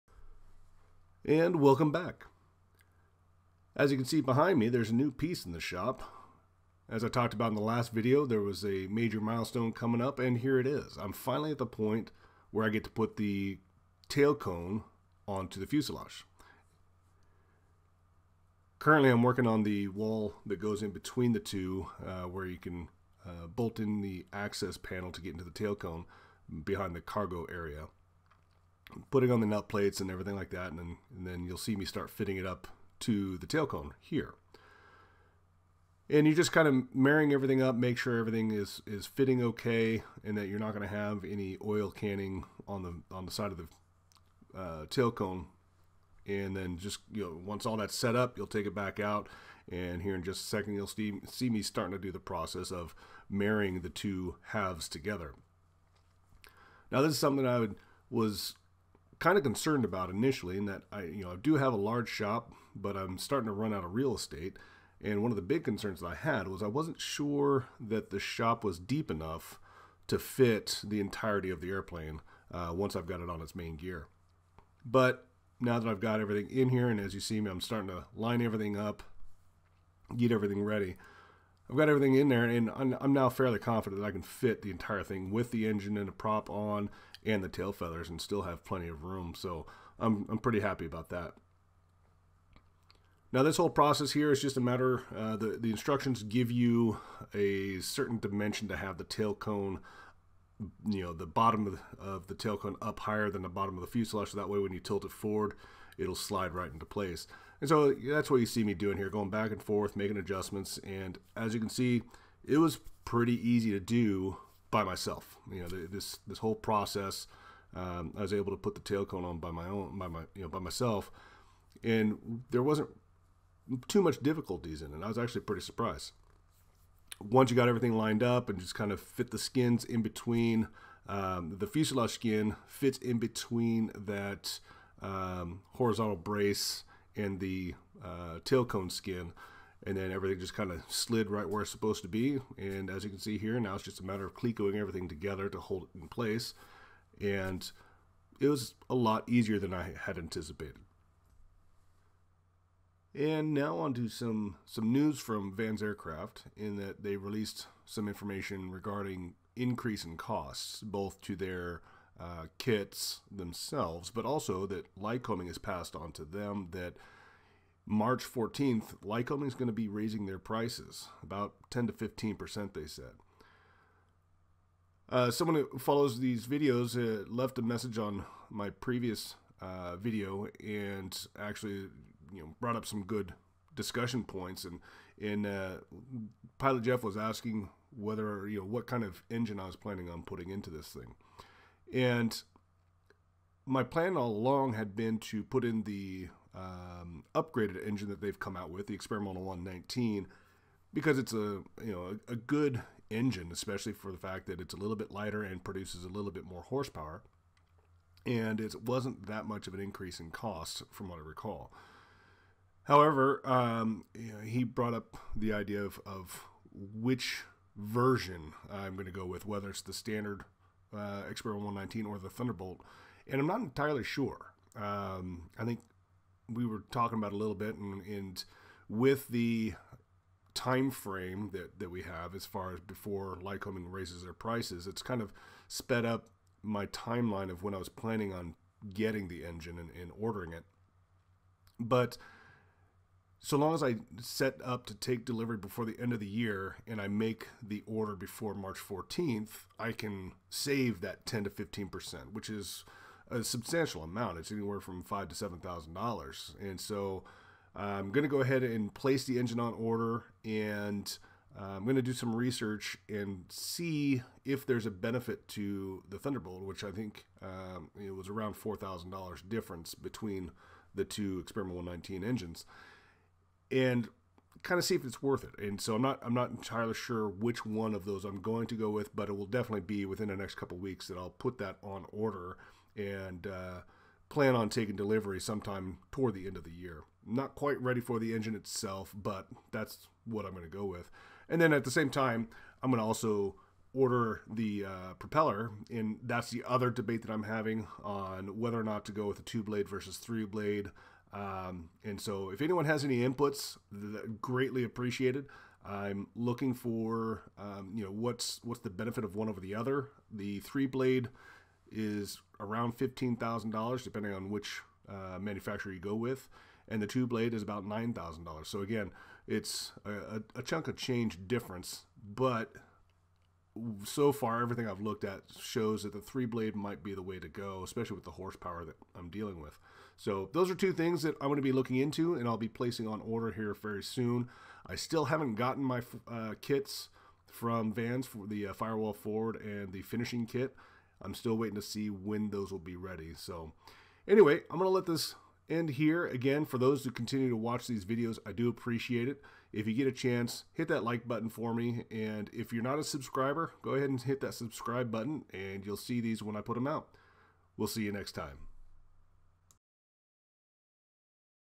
Scott's RV14a - And the Two shall become One
Finally combining the fuselage and the tail cone! Airplane noises coming soon!!!